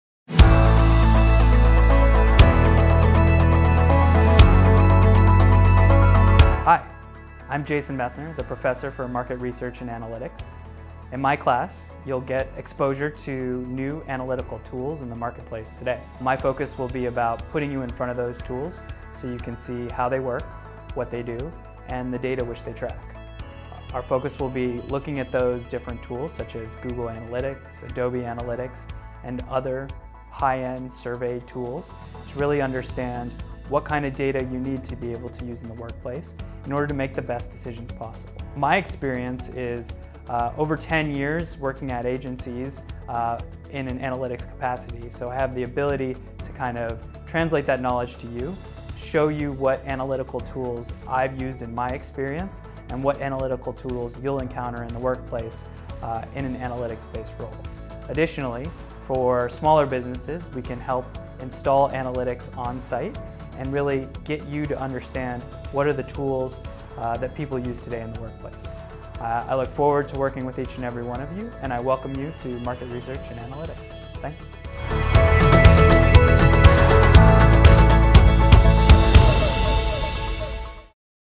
Market Research and Analytics with Jason Methner | SDSU College of Extended Studies
Learn how to differentiate your website and product for a competitive edge in customer experience. This class will cover the principles of market research and analytical tools used to analyze site behavior and identify consumer insights through marketing trends. Through hands-on, experiential learning, you will use digital tools in the Google Suite, understand the digital tool landscape and explore digital behavior through this five-week course.